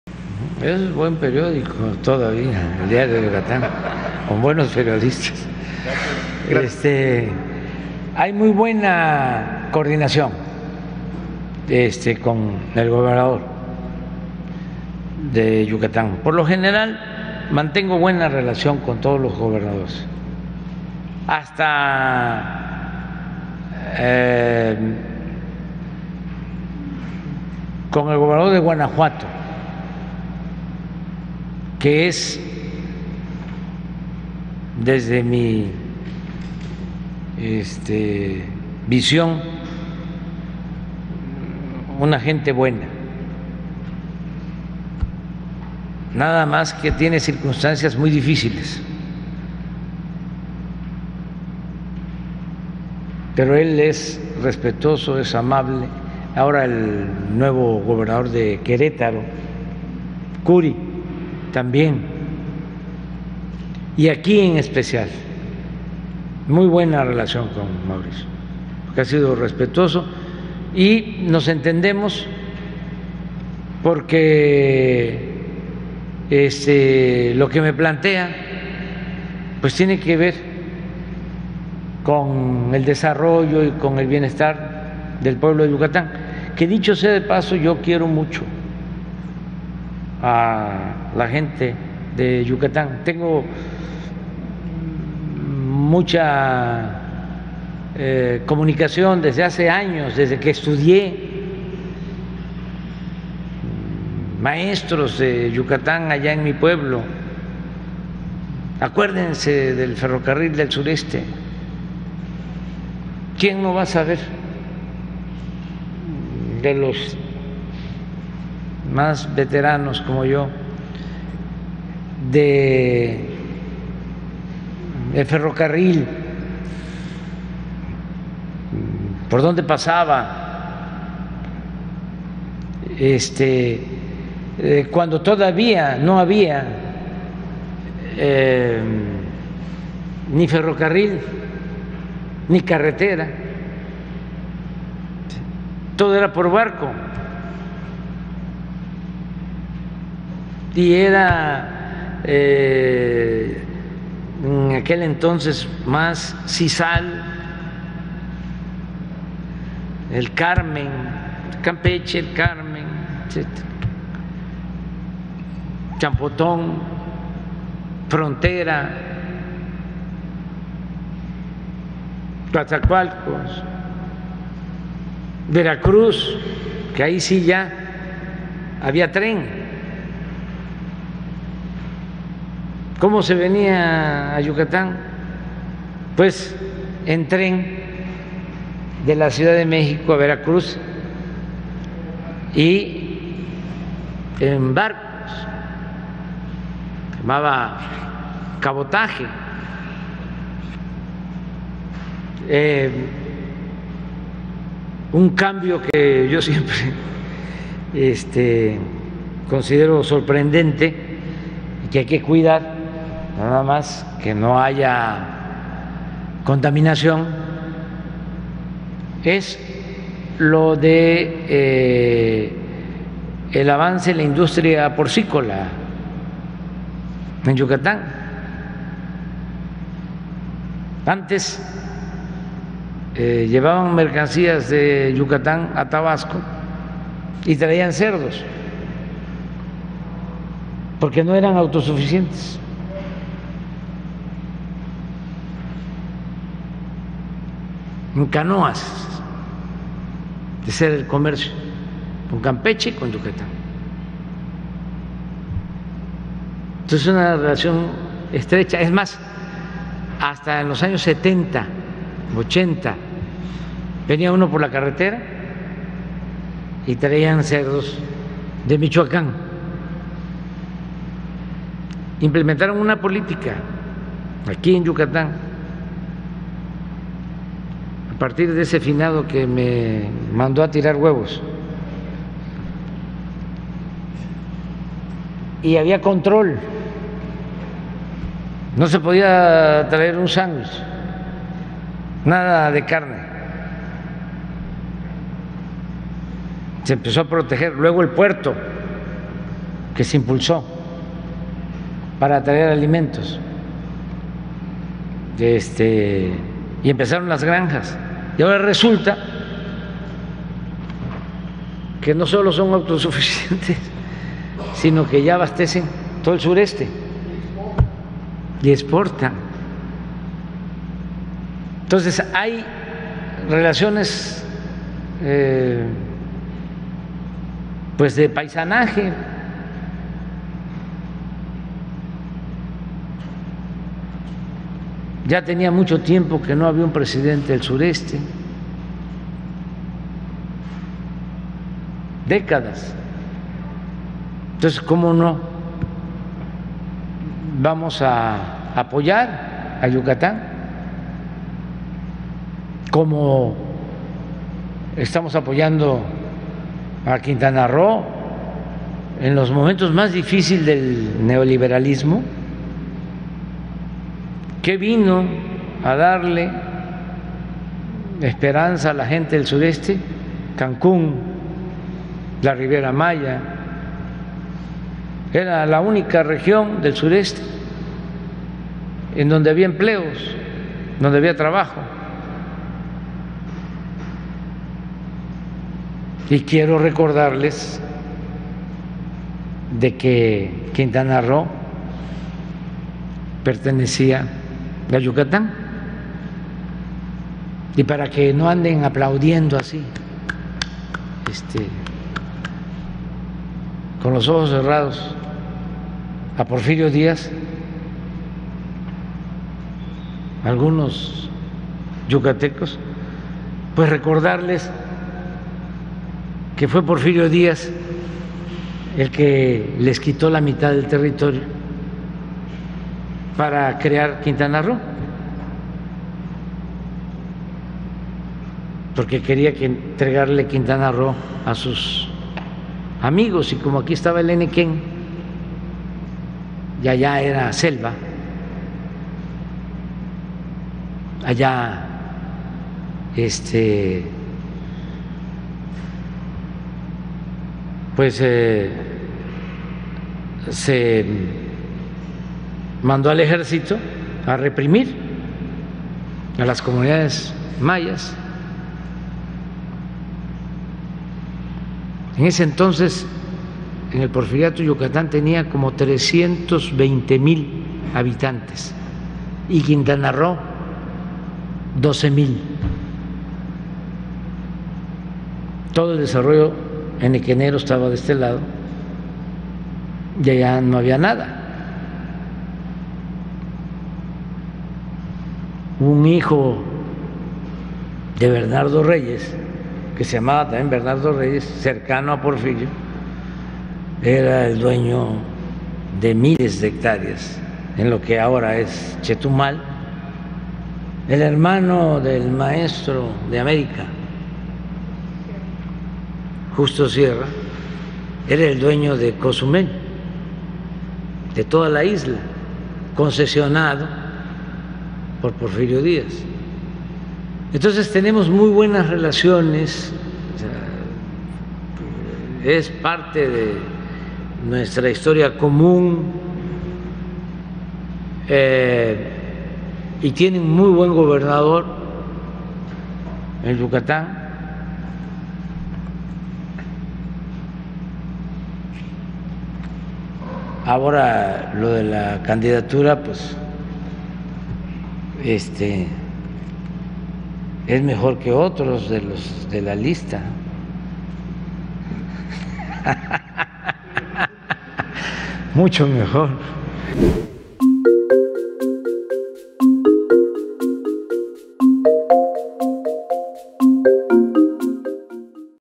Mauricio Vila es "mucho mejor" que otros presidenciables de la oposición: AMLO
El Presidente aseguró que tiene buena relación con el gobernador de Yucatán, asimismo con Diego Sinuhé y Mauricio Kuri.